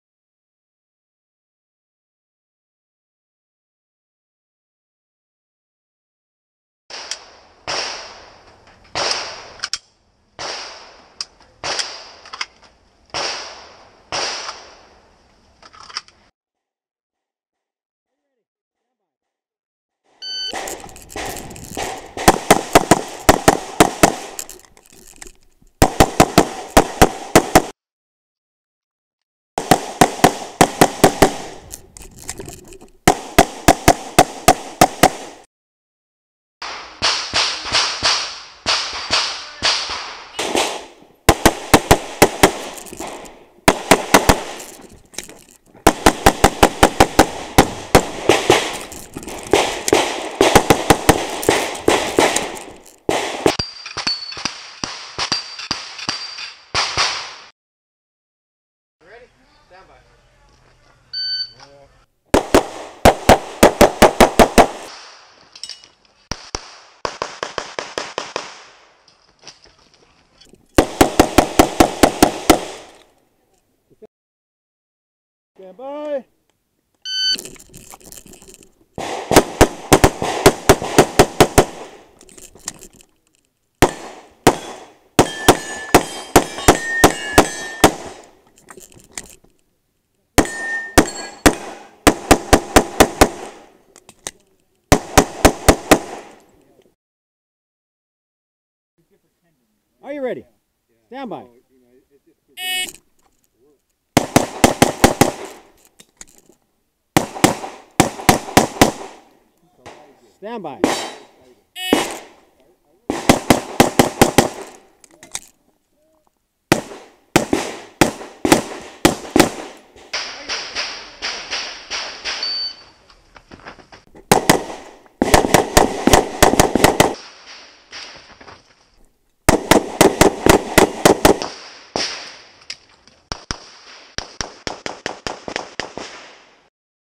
SIG P320 X5 - Grayguns Enhanced Lever Trigger in Competition
Yong Lee (4 division USPSA Grand Master) shooting SIG P320 X5 with Grayguns Enhanced Lever Trigger. USPSA Marysville 6-18-17